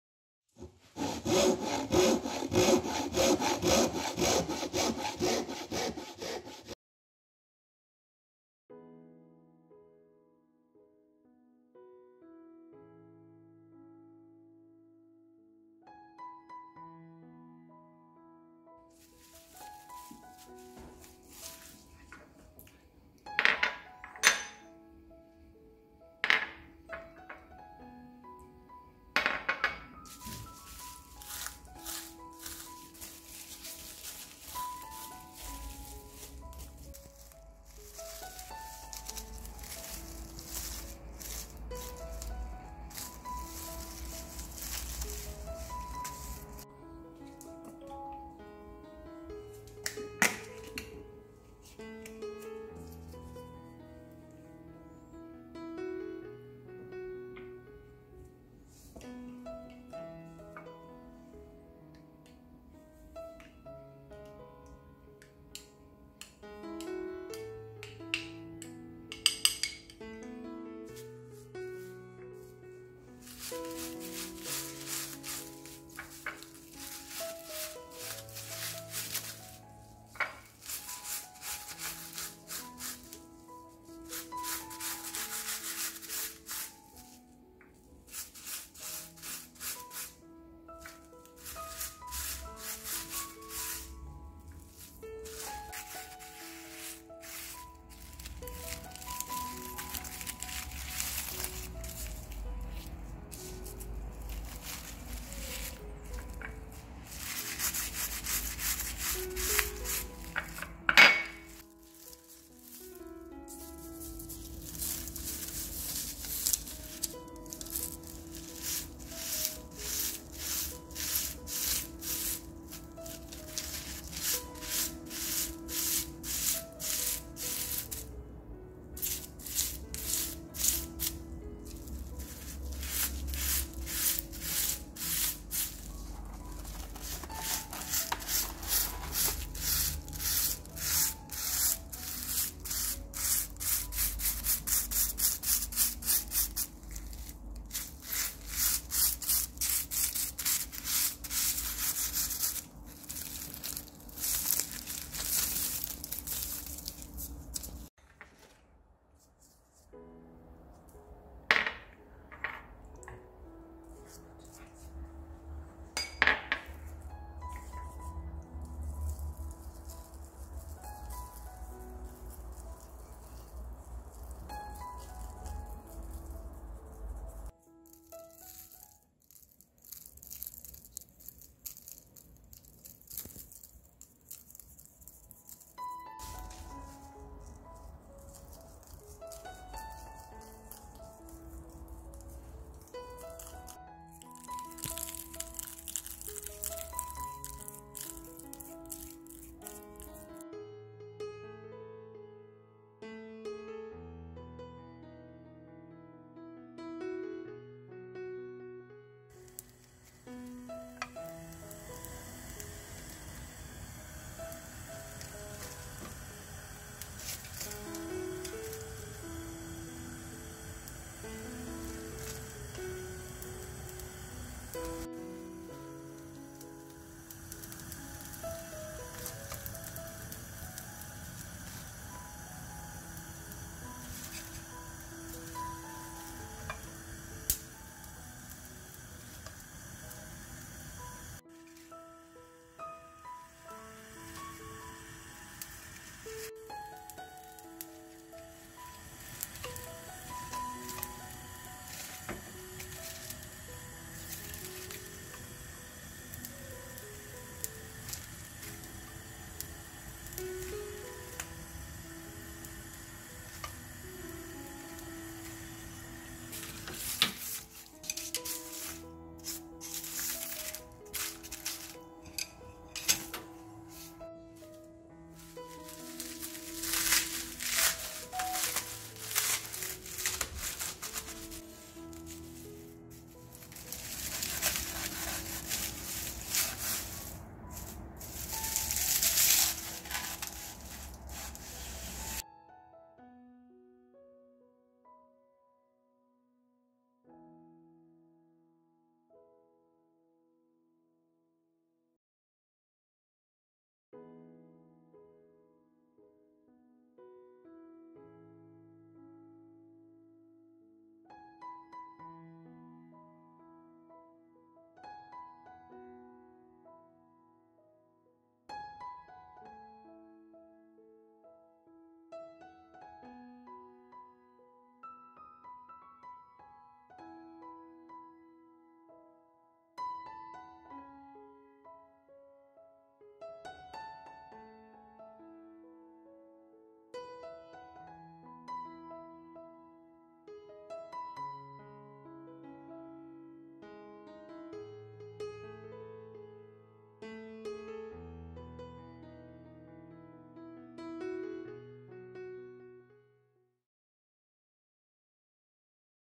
엄마손맛 고소한 김굽기 - 플레이팅/ Korea Laver (Food&Wood)
어머님이 만들어 주셨던 그 맛!!

시중에 파는 조미김 보다

조금은 귀찮치만 맛있게 두고 두고

먹을 수 있는 김구이!

woodcarving
Joinery
woodturning